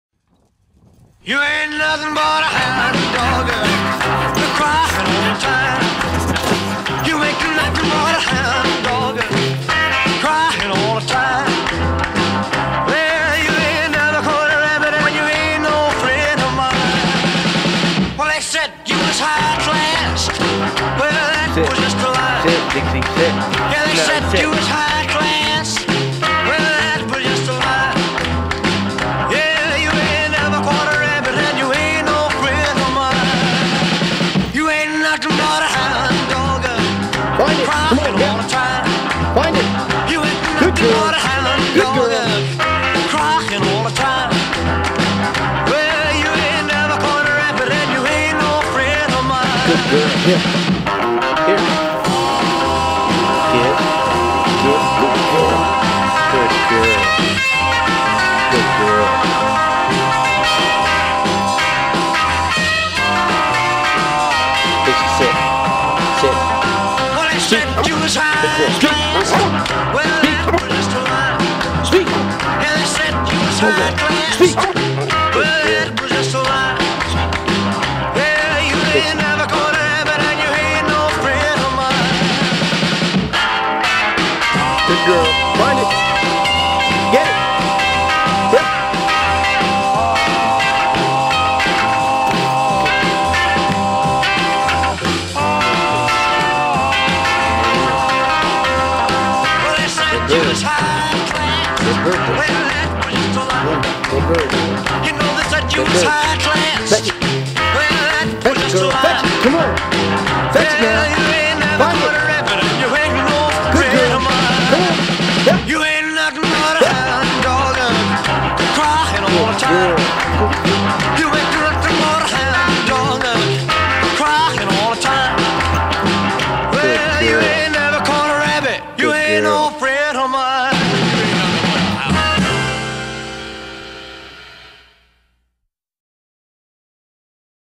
Dixie Lee Samples of Brogue
SN883101/01 (AKC Registration)
LR-159858E53F-NOPI (OFA-hips)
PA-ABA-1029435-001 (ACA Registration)

Dixie has been raised as a family pet and gentle companion.
Dixie is great around children and has a wonderful temperament.
Dixie is a strong retriever and loves to hunt.